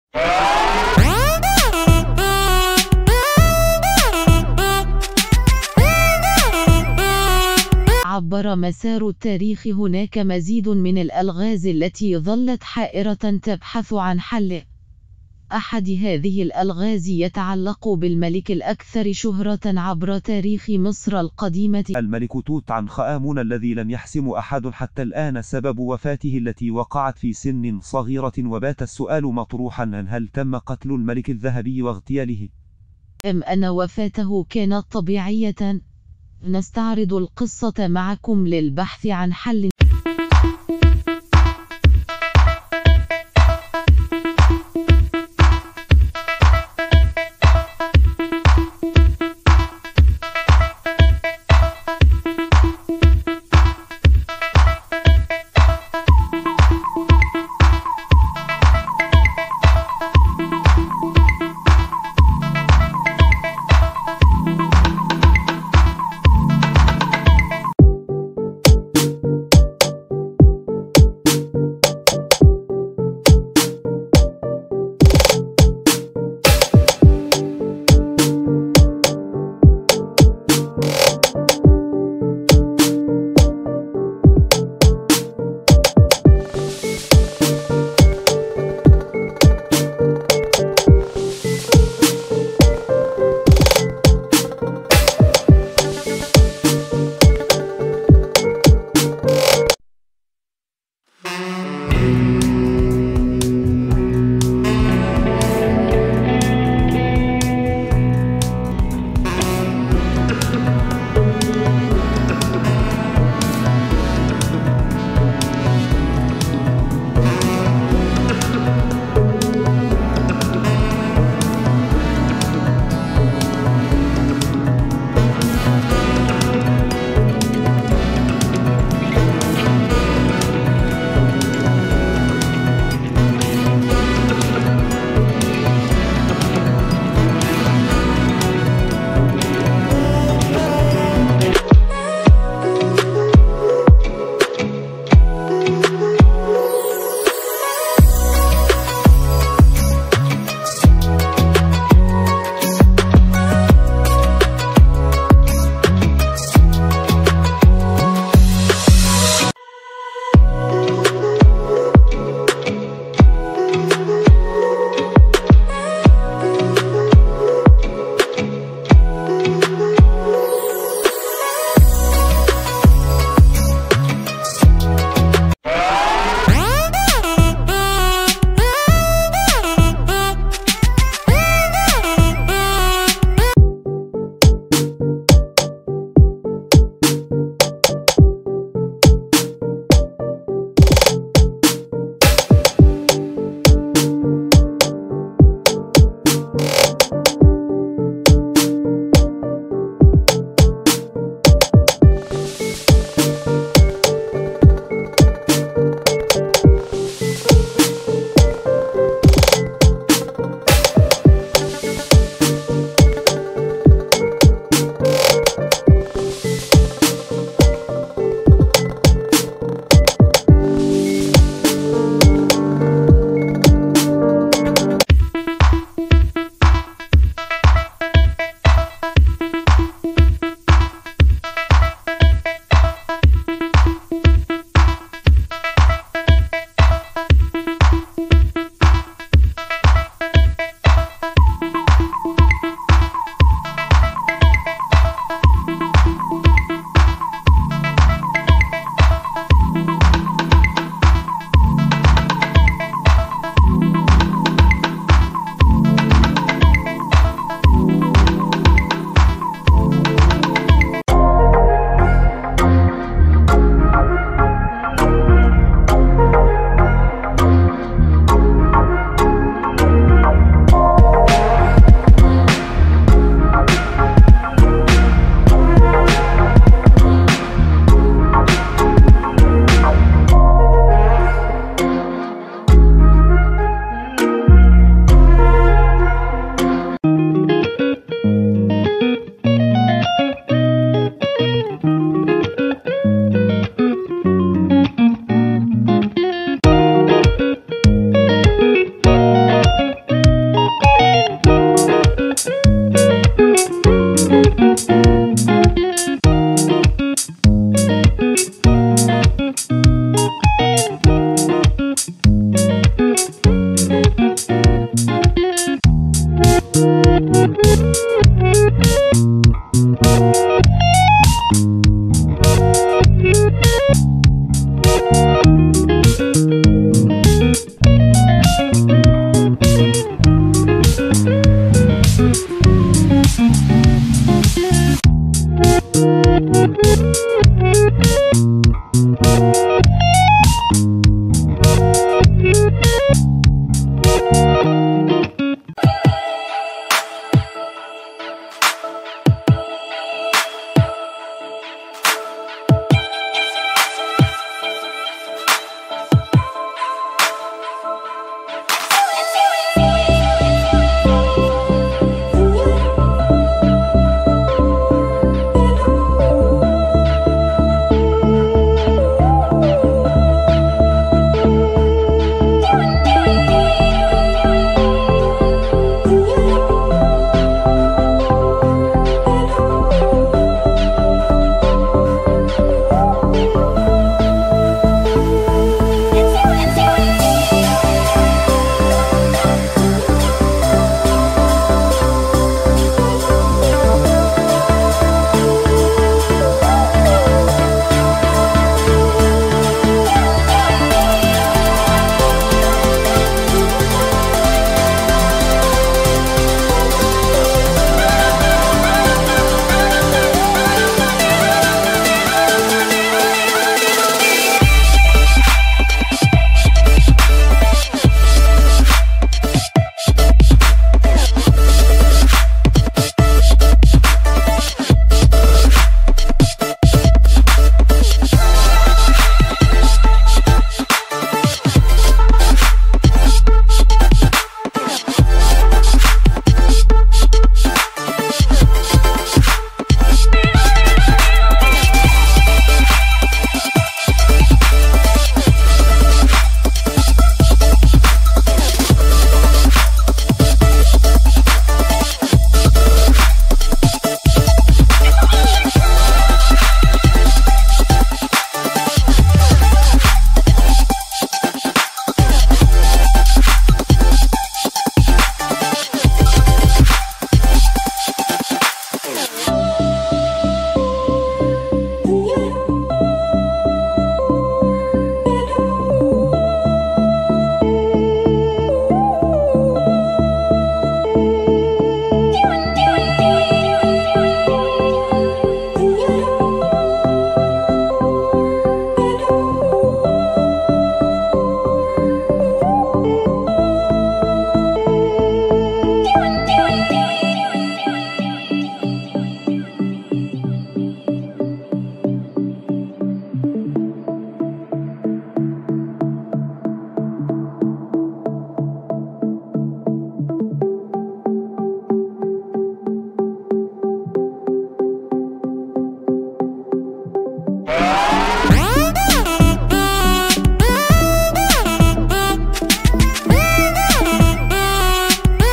مين إللي قتل توت_عنخ_آمون؟..لغز حائر بلا حل.إنتاج
عبر مسار التاريخ هناك مزيد من الألغاز التي ظلت حائرة تبحث عن حل..أحد هذه الألغاز يتعلق بالملك الأكثر شهرة عبر تاريخ مصر القديمة..المَلِكُ توت عَنخ آمون الذي لم يحْسِمُ أحدٌ حتي الآن سبب وفاته التي وقعت في سن صغيرة وبات السؤال مطروحاً هل تم قتل الملك الذهبي واغتياله؟..ام أن وفاته كانت طبيعية؟..نستعرض القصة معكم للبحث عن حل.